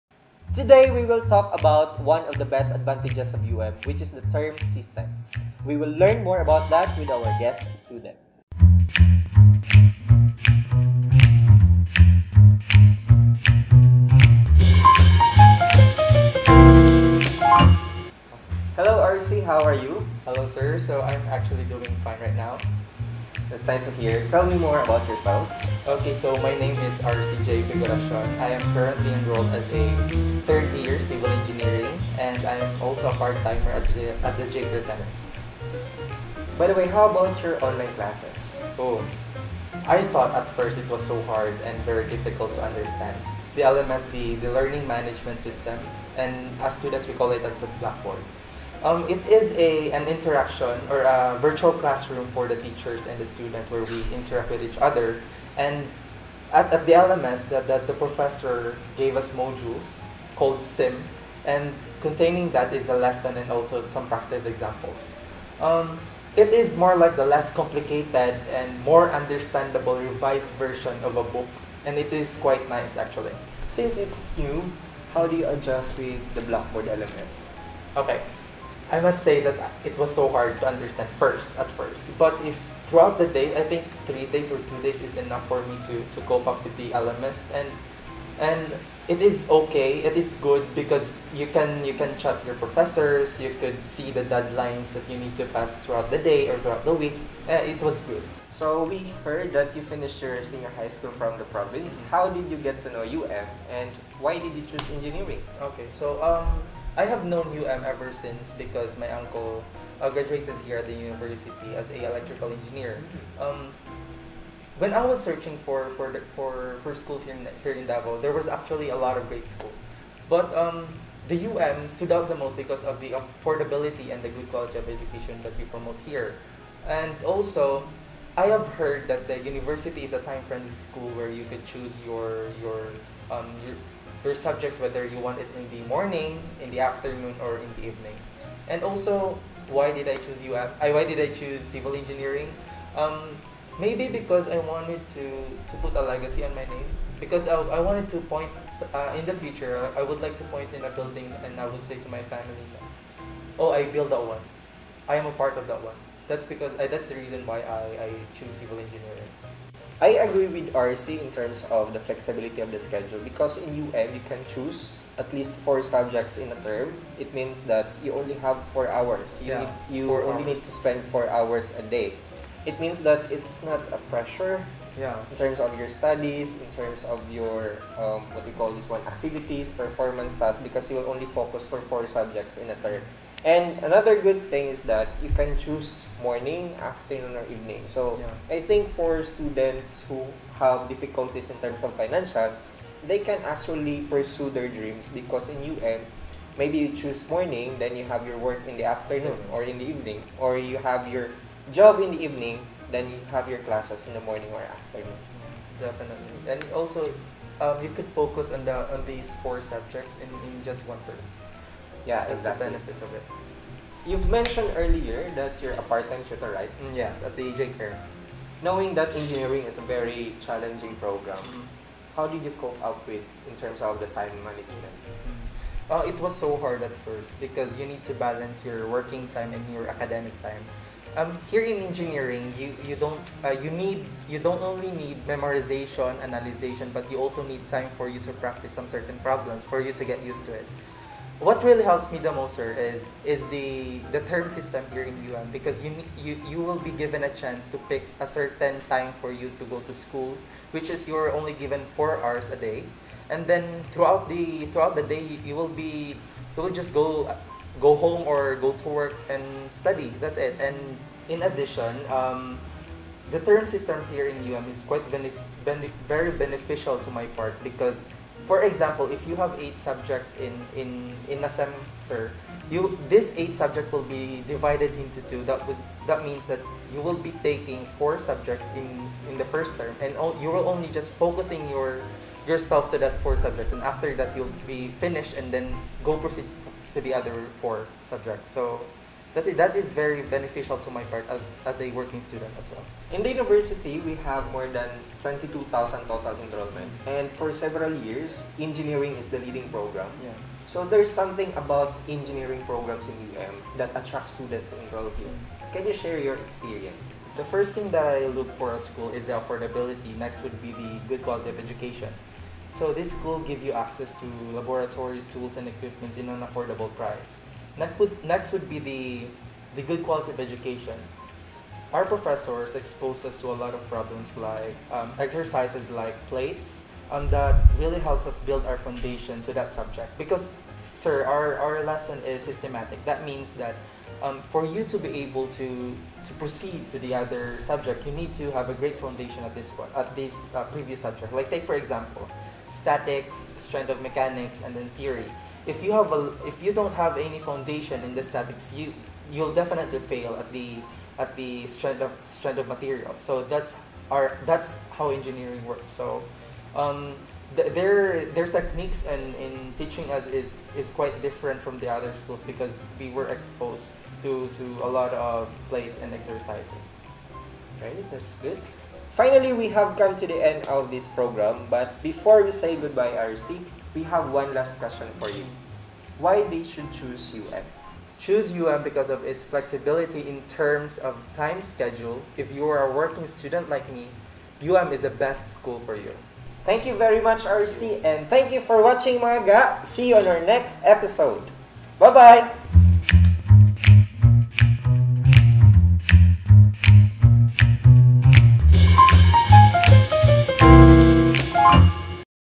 What's In UM? - (Episode 4) Balancing WORK + SCHOOL as a UM Engineering Student
RC Jay Figuracion shares about UM's term system and flexible schedule that help him balances WORK as a tutor and SCHOOL as an engineering student.

Learn more about his experiences by watching this video...